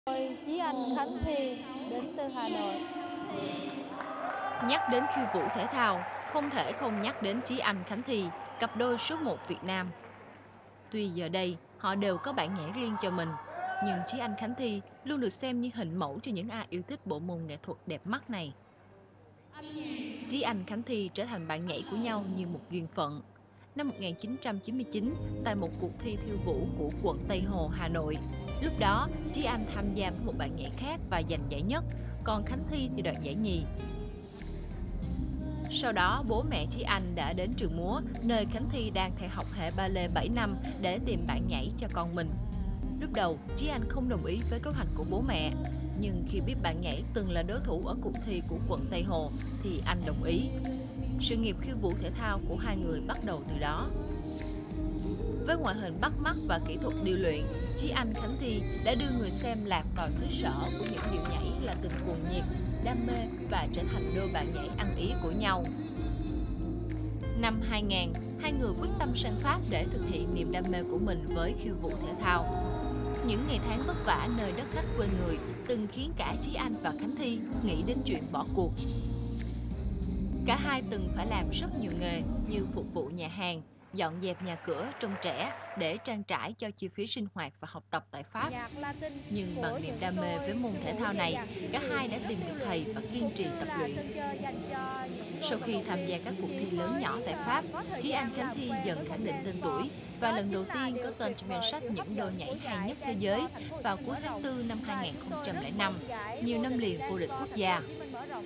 Chí Anh - Khánh Thi (Rumba) | Cặp đôi vàng của nền Khiêu Vũ Thể Thao Việt Nam
Clip được quay trong giải vô địch khiêu vũ thể thao TPHCM năm 2008